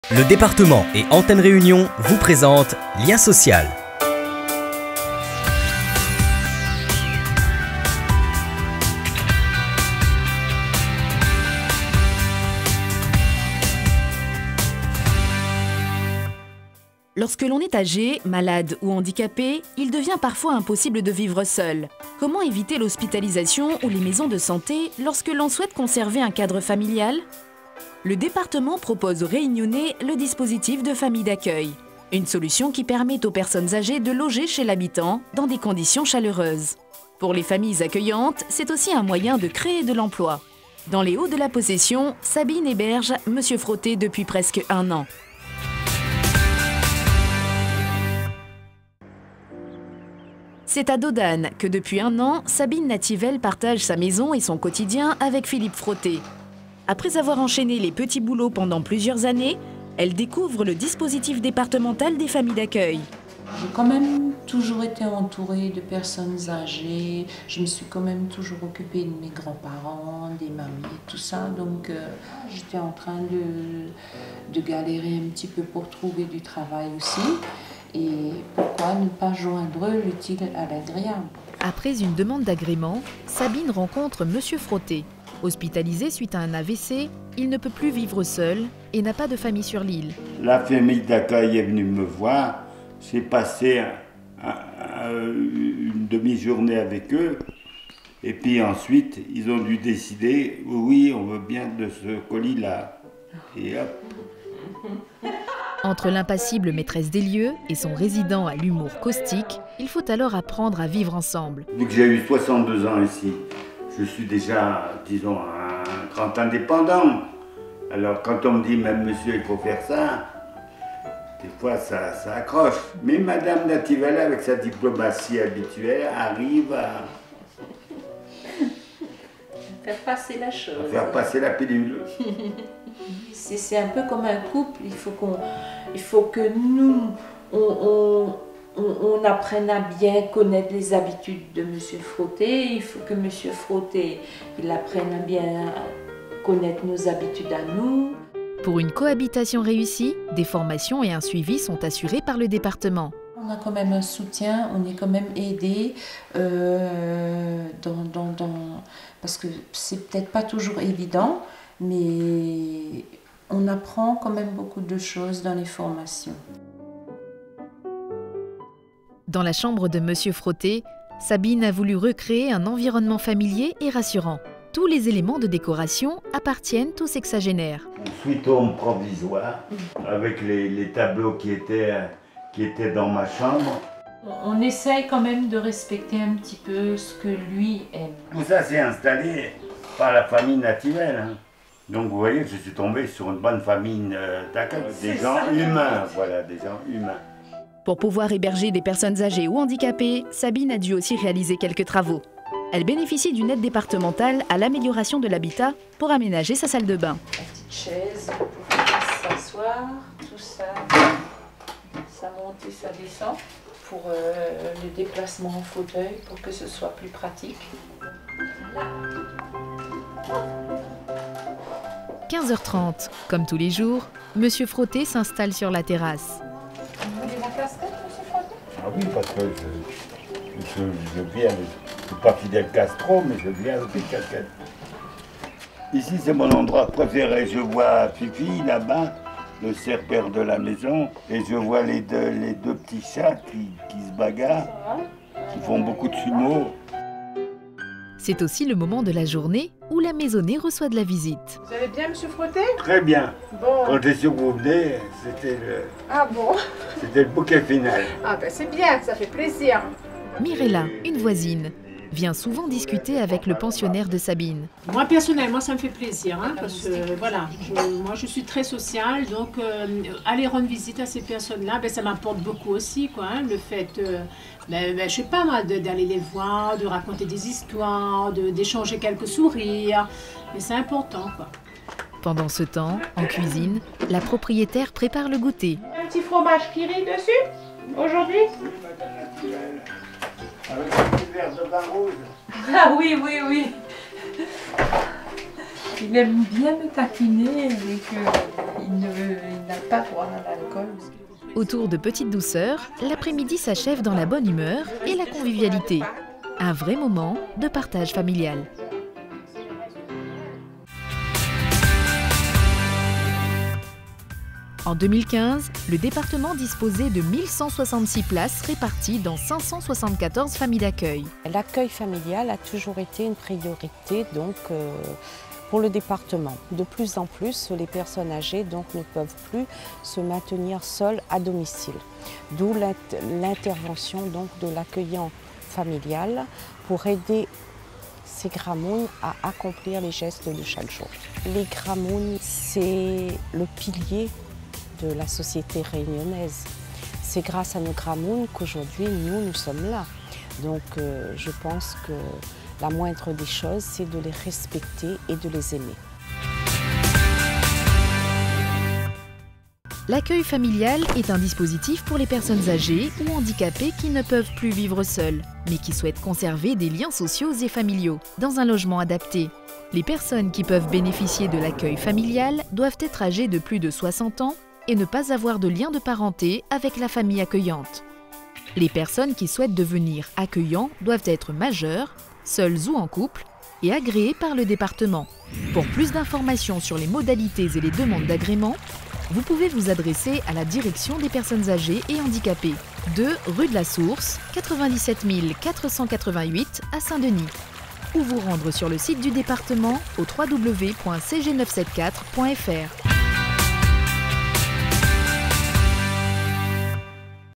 Lien Social 2016 - Famille Accueil
Les familles d'accueil hébergent chez elle des personnes âgées, malades ou handicapées. Ce dispositif départemental a pour objectif de proposer à ce public fragilisé un cadre de vie chaleureux et agréable, ou les liens sociaux sont maintenus.Dans les hauts de la Possession, Sabine, accueillante familiale, héberge M. Froté depuis presque un an.